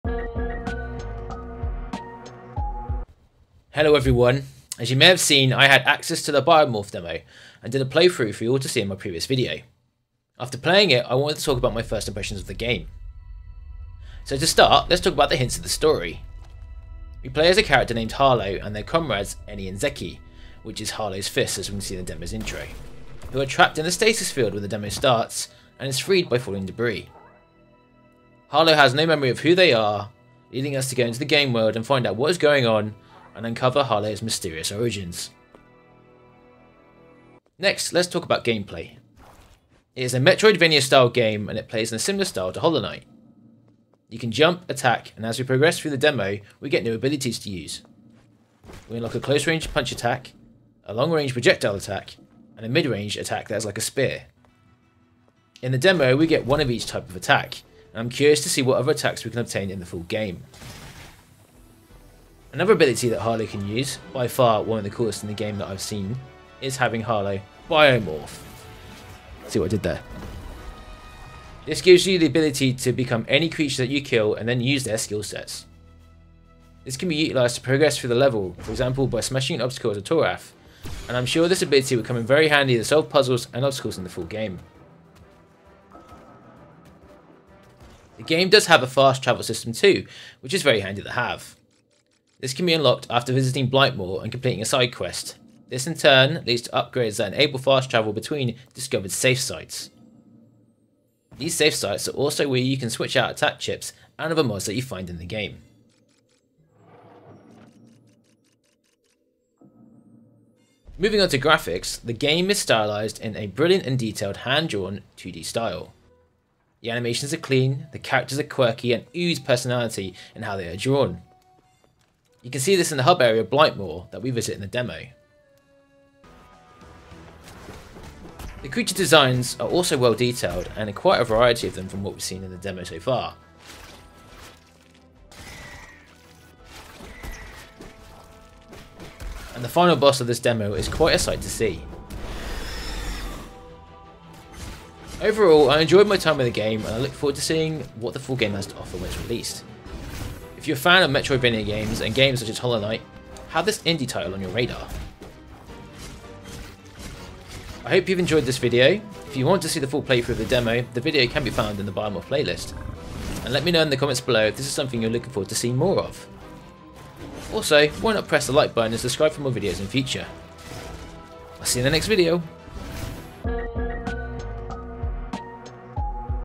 BIOMORPH! MY FIRST IMPRESSIONS!
Hello everyone and we are back with a follow up video to BIOMORPH! This time I am talking about my first impressions of the game from the demo that I was kindly given by Lucid Dreams Studio, many thanks again to them for the opportunity. Lets jump straight into it shall we!

(Gifted)